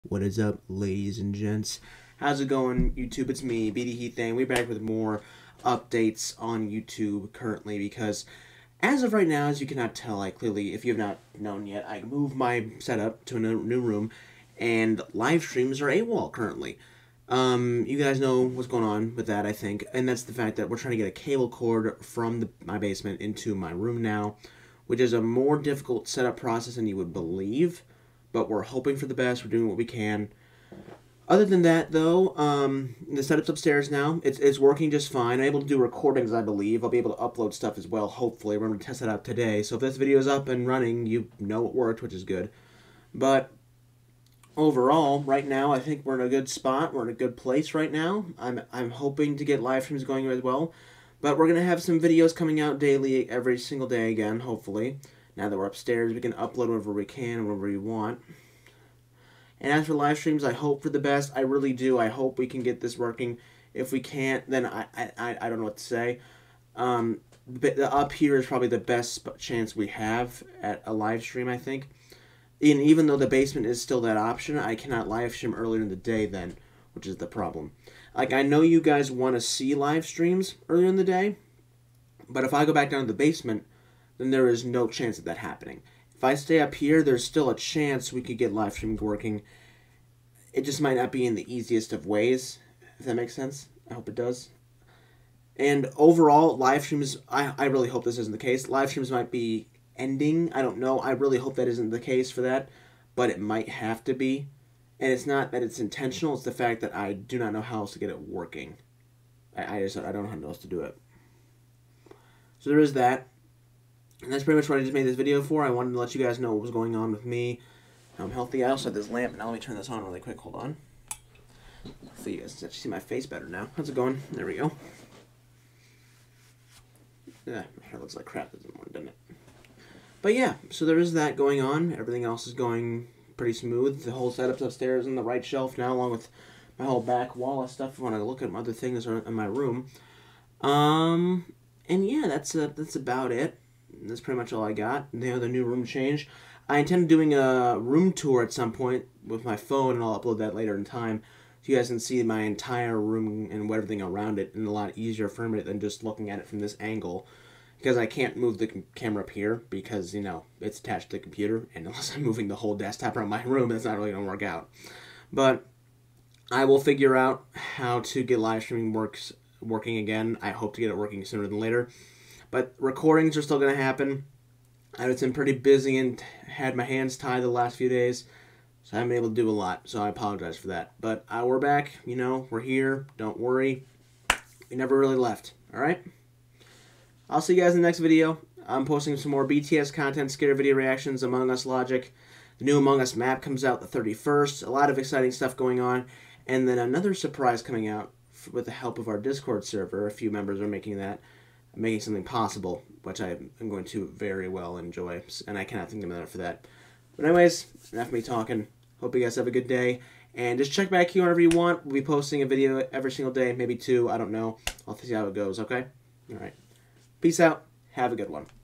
Update on Room Change and Live Streams
Hey everyone! Just wanted to check back in to let you all know that I'm alright and working on fixing the lag in live streams. That and school so I hope you all understand that we're trying to work around the problems.